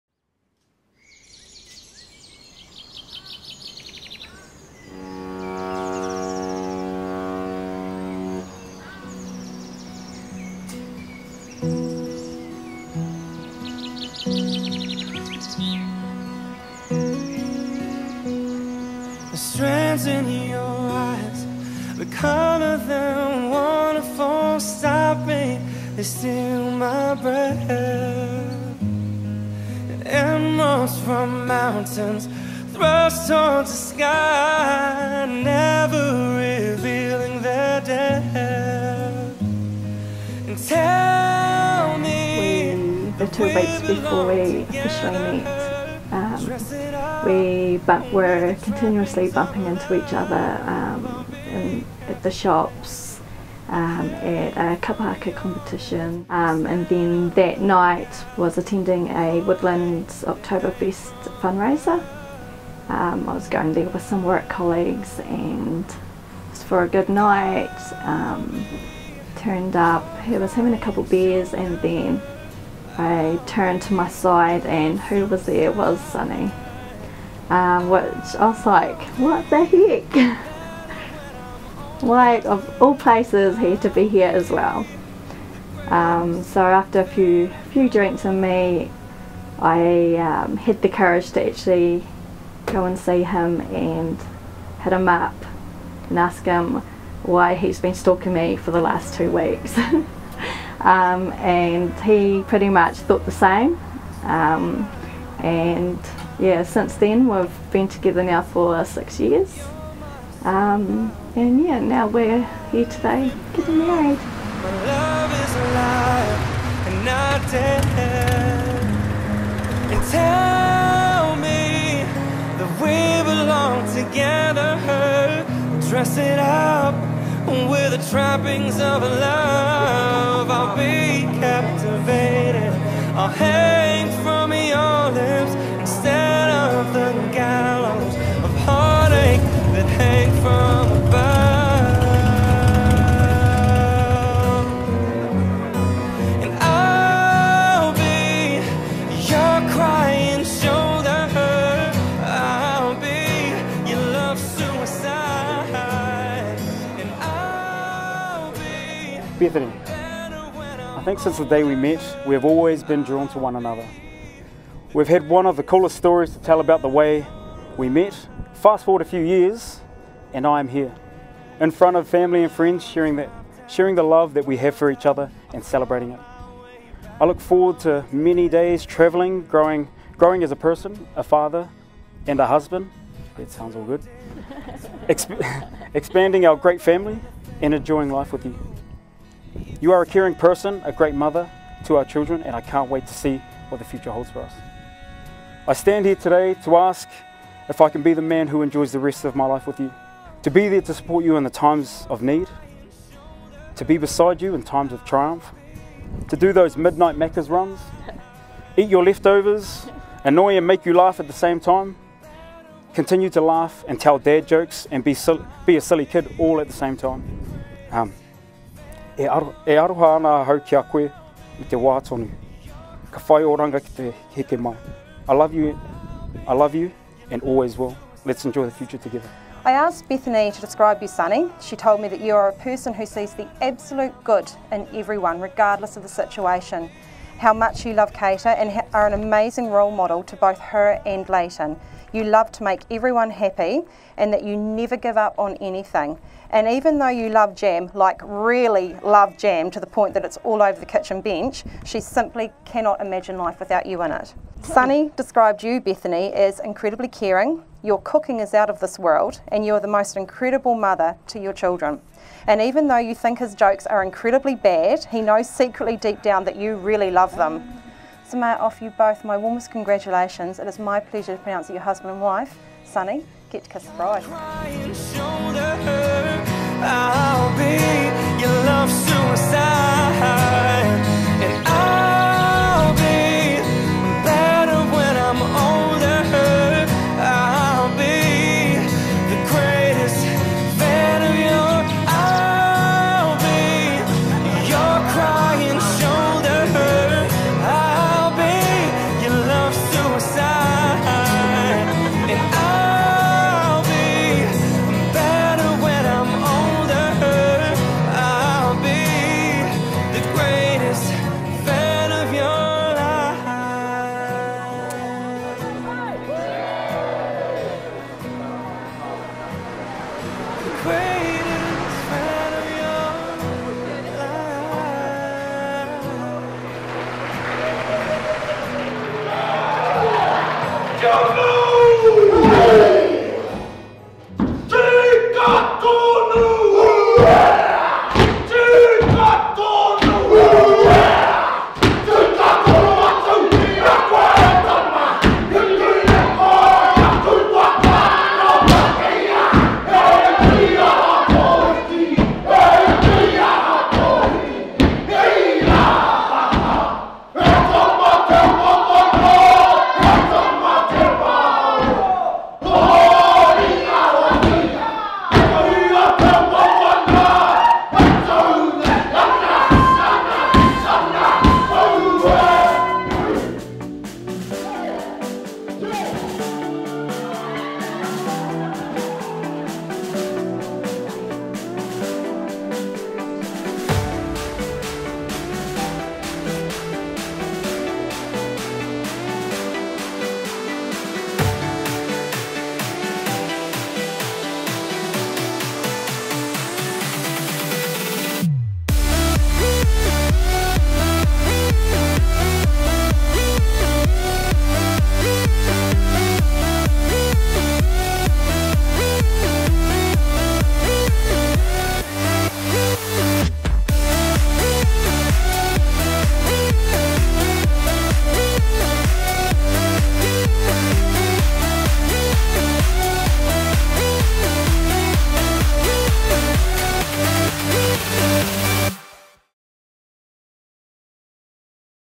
BETHANY & SONNY (NZ High Country Wedding, Queenstown, New Zealand)
A stunning wedding ceremony at NZ High Country Wedding in Queenstown, New Zealand.

If you are getting married in Queenstown or Wanaka we would love to chat about filming your wedding! Get in touch, tell us about your plans and get a free quote in less than 24 hours.